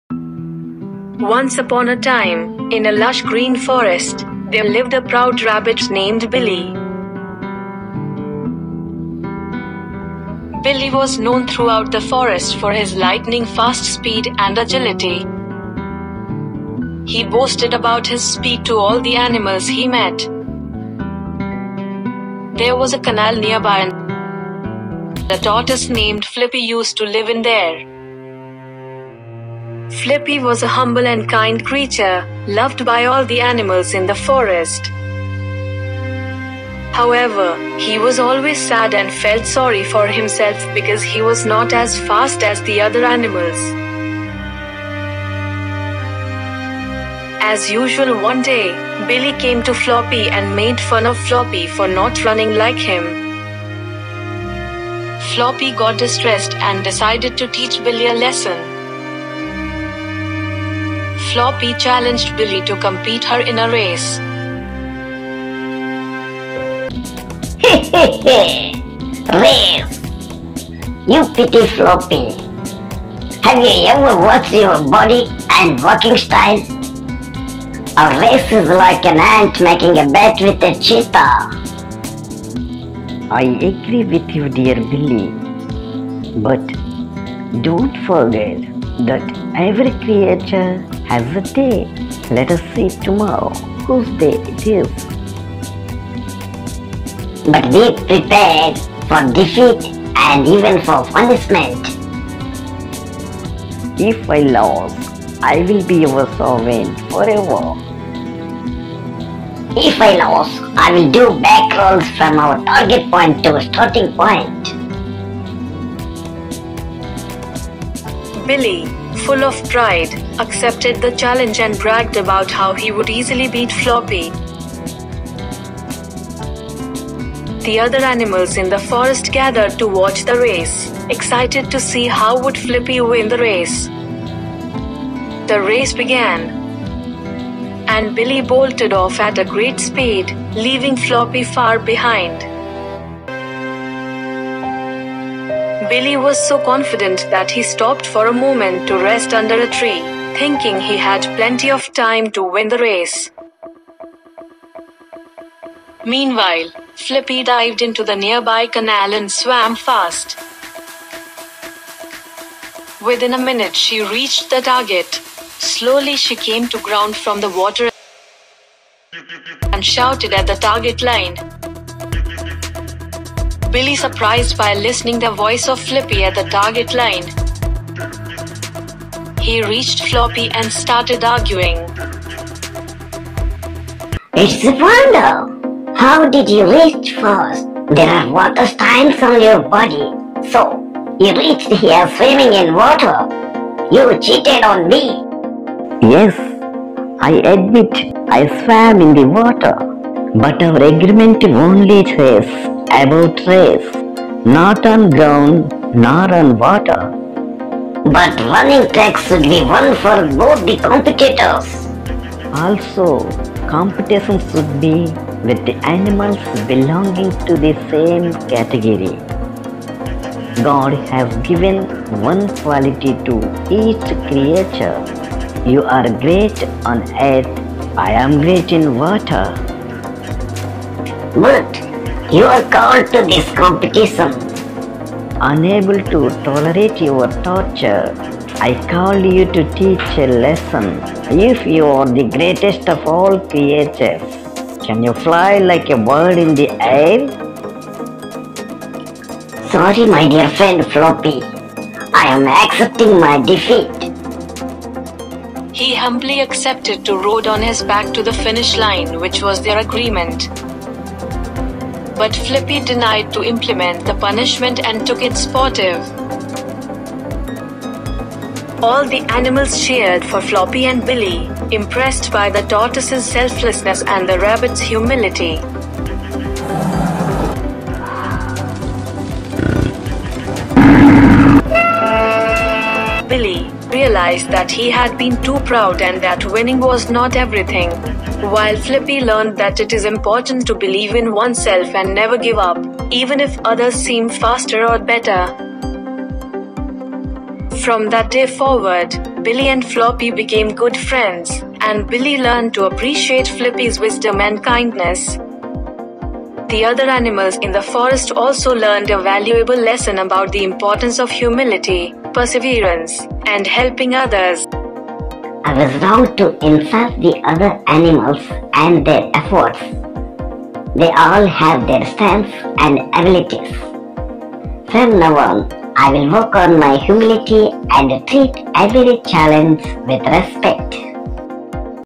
The Egoist Rabbit and the Wise Tortoise: A Lesson on Humility
@naatustories Description: In this animated short story, we follow the race between an egoist rabbit and a wise tortoise. The rabbit, confident in his speed, takes a break before crossing the finish line, thinking the tortoise won't catch up. But the tortoise takes a water route and wins the race. After the race, the tortoise shares a valuable lesson on humility and embracing one's unique qualities. This story teaches children and adults alike to appreciate the strengths and weaknesses of others and to avoid underestimating anyone based on their appearance or perceived limitations.